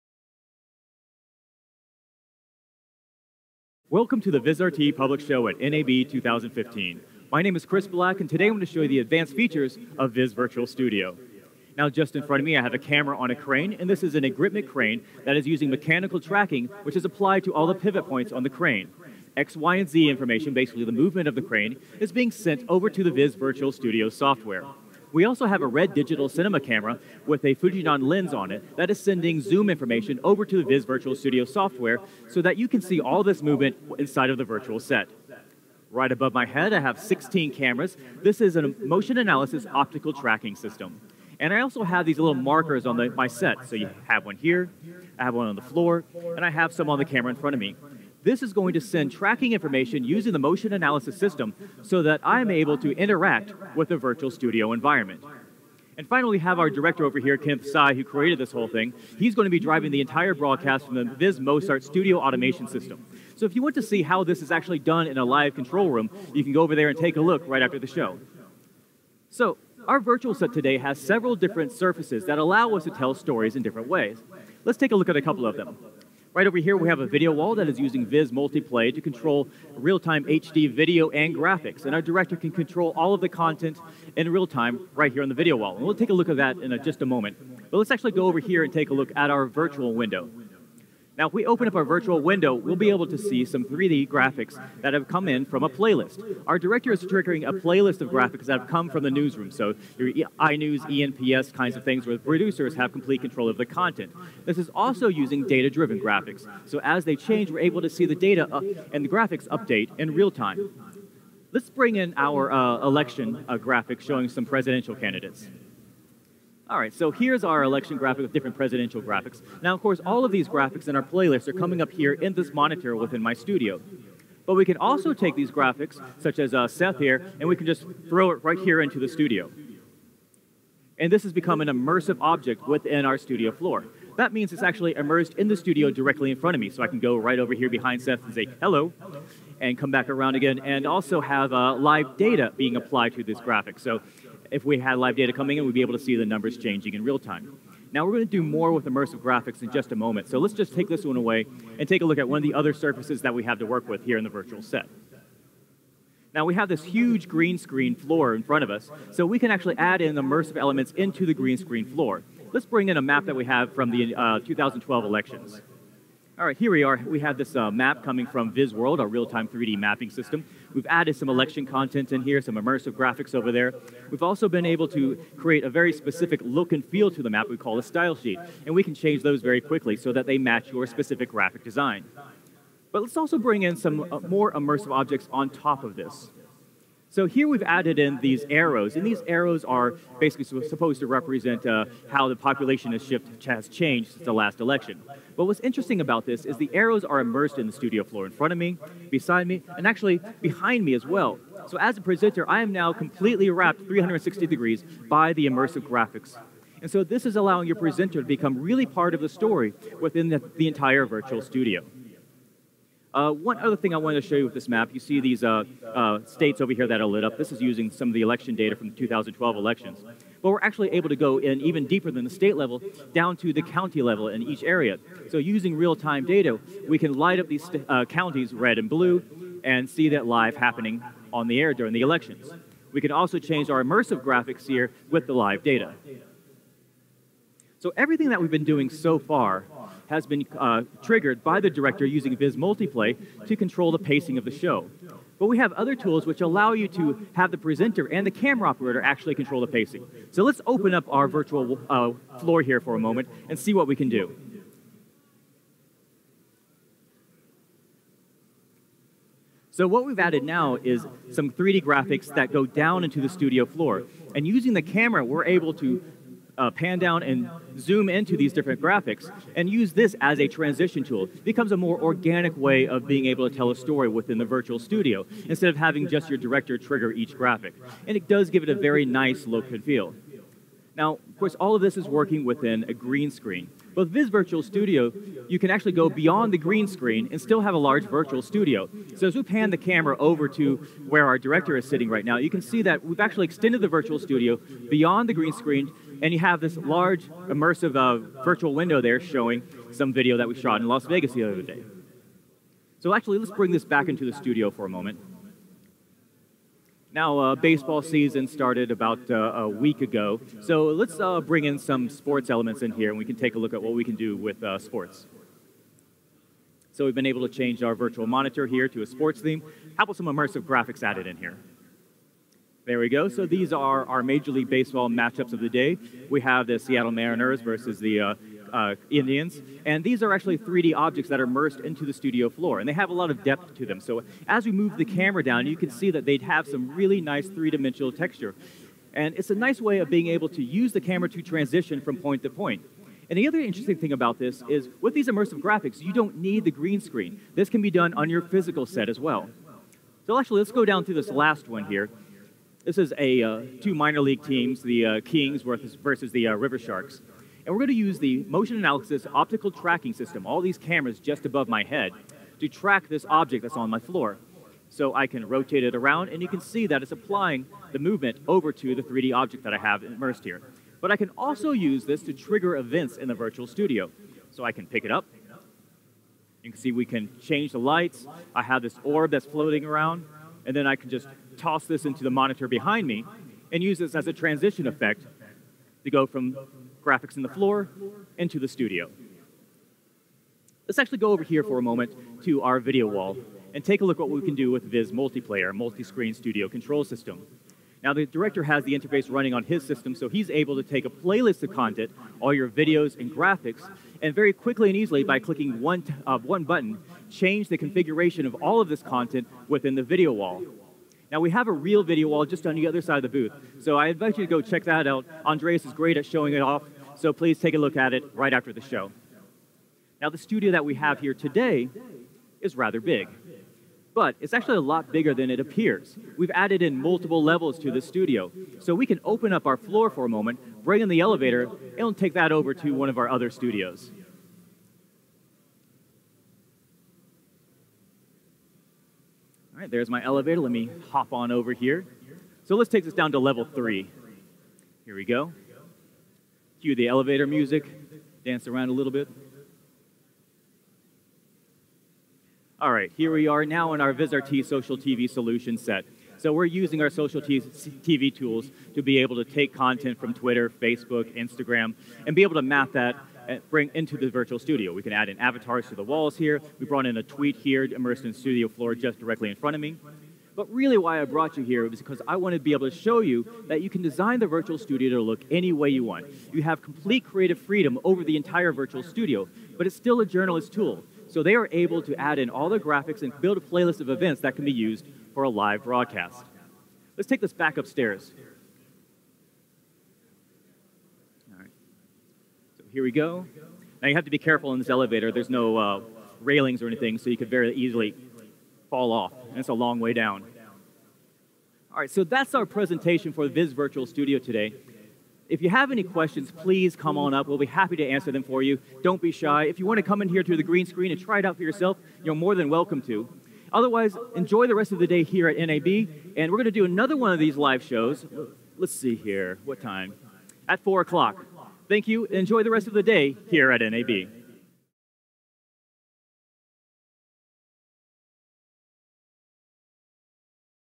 [Vizrt] 2015 NAB Vizrt 虛擬棚
Vizrt 虛擬棚可以結合不同的產品製造特殊的視覺效果。

結合Multiplay可以在虛擬棚內設計電視牆，透過NRCS電子稿單，編輯在iNews或者是ENPS輸入資料，就可以顯示在虛擬電視牆上。

結合數據整合，讓所有資料在電視牆上一目了然，讓訊息在電視牆上，或者是跳出平面的限制，成為在主播前面獨立的物件。

讓攝影機高懸，在底板上顯示選舉地圖，切換各種不同的統計資料，主播可以在攝影棚內走動，而不是單板的只在固定的位置播報。

除了往上發展，我們也可以設計往下發展的空間，製造不同的空間感。

Vizrt可以透過特殊的追蹤物件，讓虛擬物件追蹤主播手上的道具，改變虛擬棚的燈光或情緒，讓觀眾看到難以置信的效果。

讓觀眾更專注在螢幕前面，主播可以在一瞬間從明亮的虛擬場景轉移到截然不同的虛擬場景，而這一切變化都只需要在動畫內製作完成。